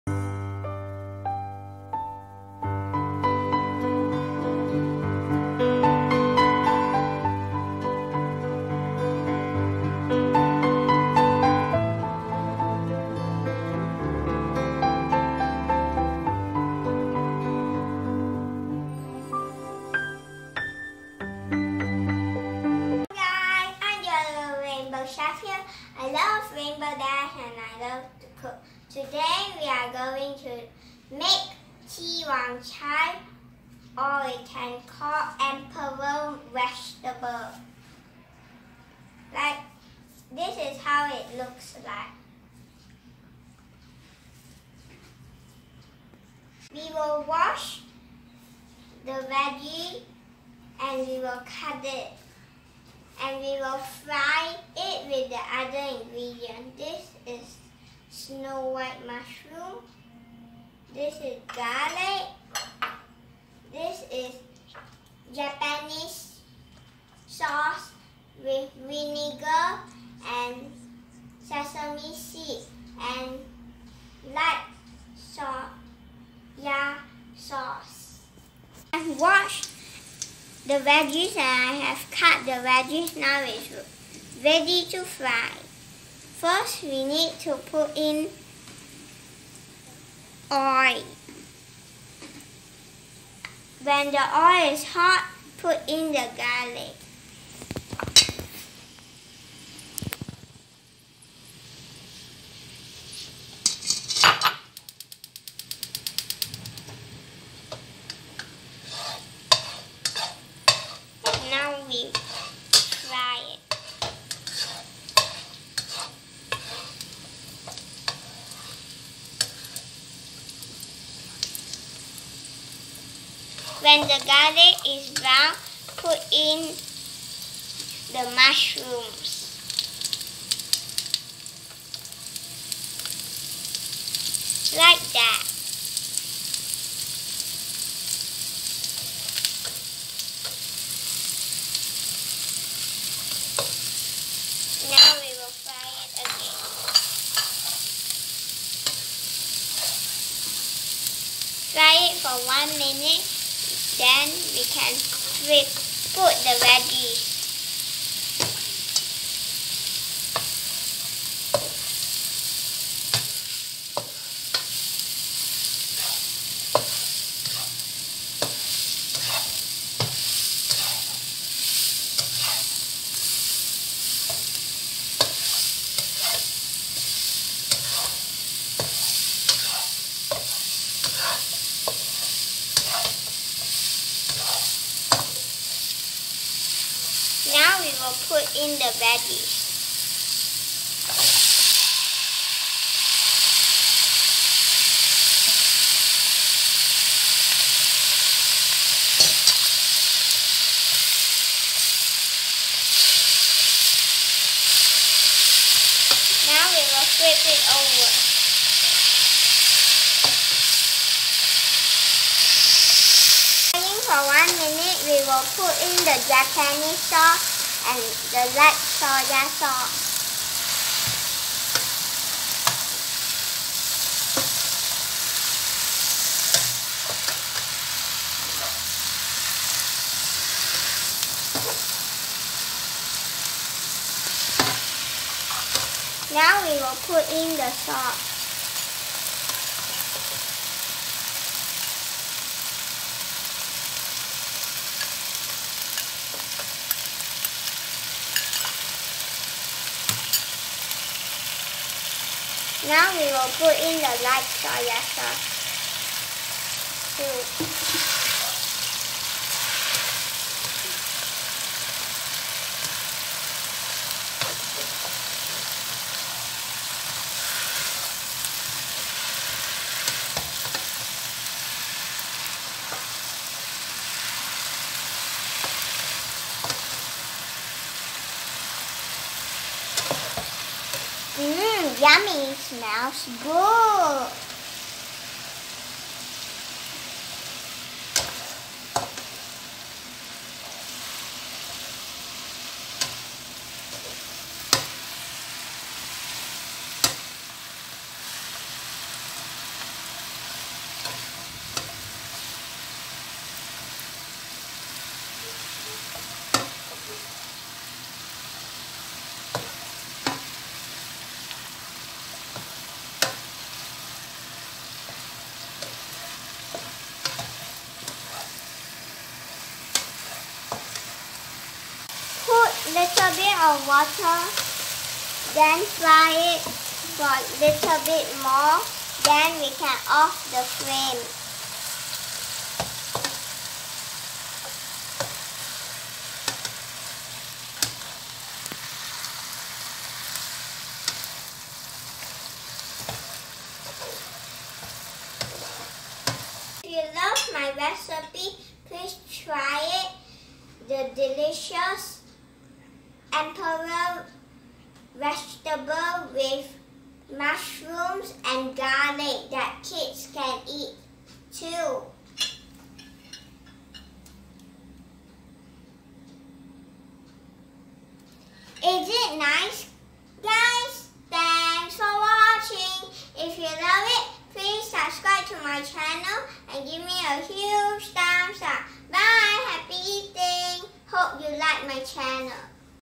Ti Wang Cai Emperor Vegetable in Japanese Sauce
JoJo stir frys a green leafy vegetable known as Emperor Vegetable or Ti Wang Cai.  Try it and enjoy!:)